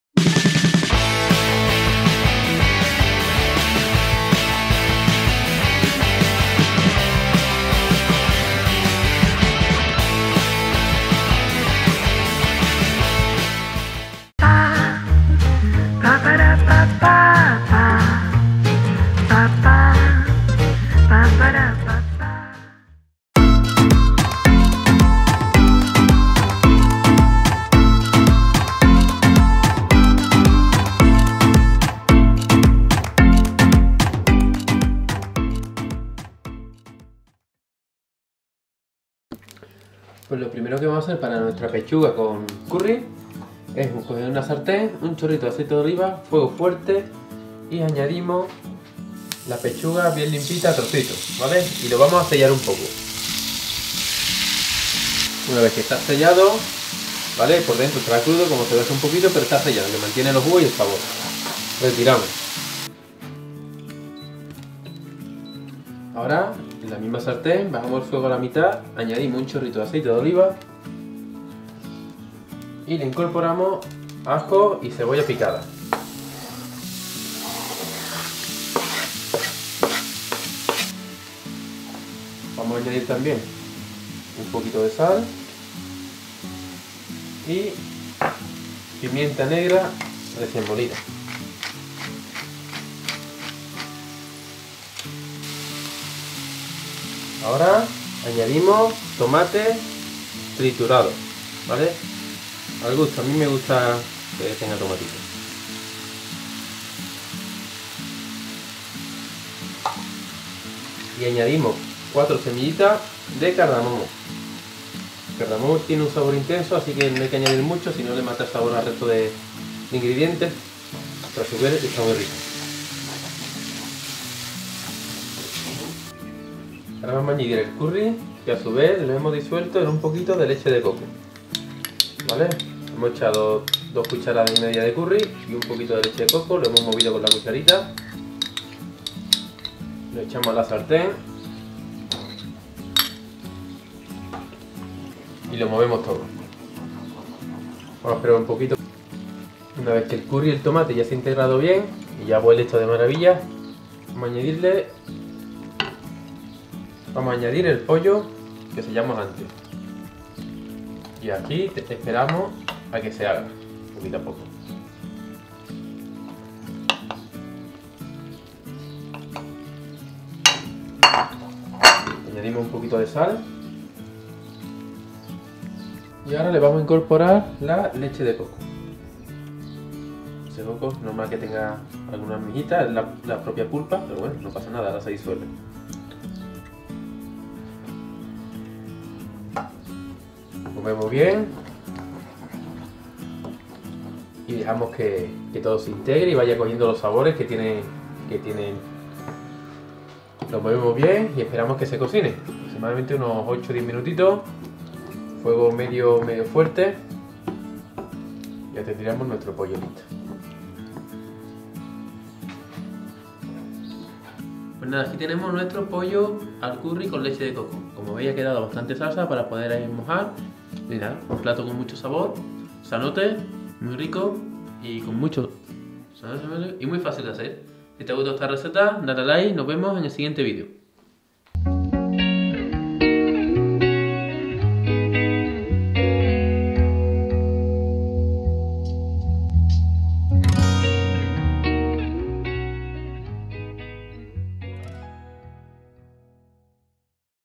POLLO AL CURRY CON LECHE DE COCO
Pollo al curry con leche de coco. Receta muy jugosa y rápida de hacer
Ingredientes:
Pollo          300gr.
Tomate natural: 200 gr
Ajo: 2 dientes.
Cebolla: media cebolla.
Cardamomo: 4 semillas.
Leche de coco.
Pimienta y sal.